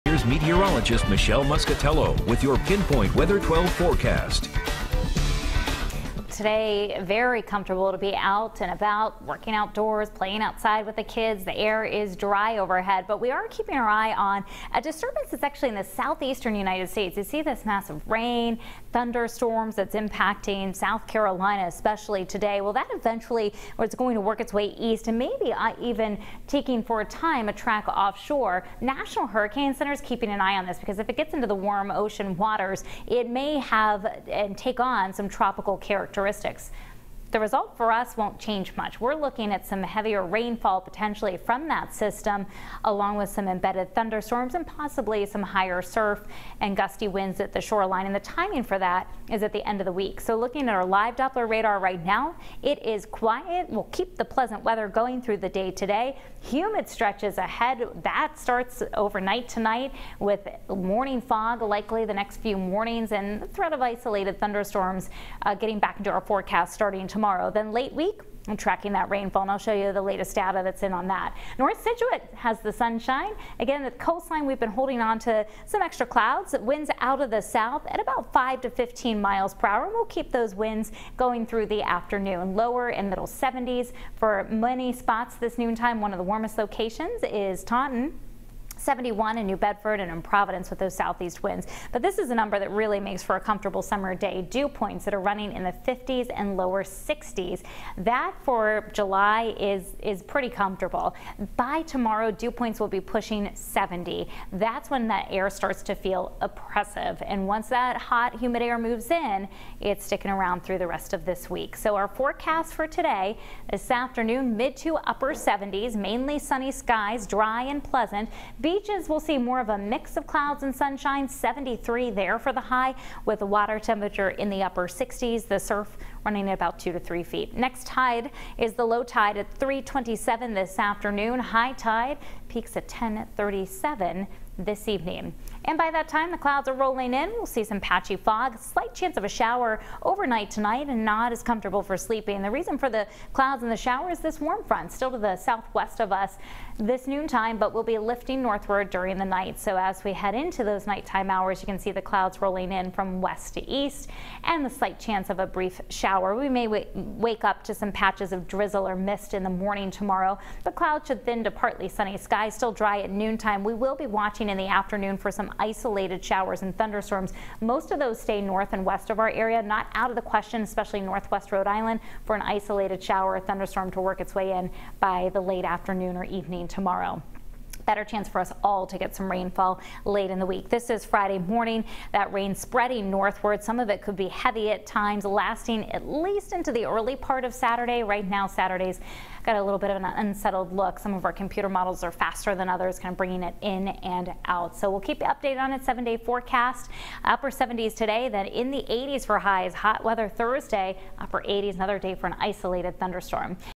Pinpoint Weather 12 Forecast
Eyewitness News Meteorologist Michelle Muscatello has the noon weather for Tuesday, July 7, 2020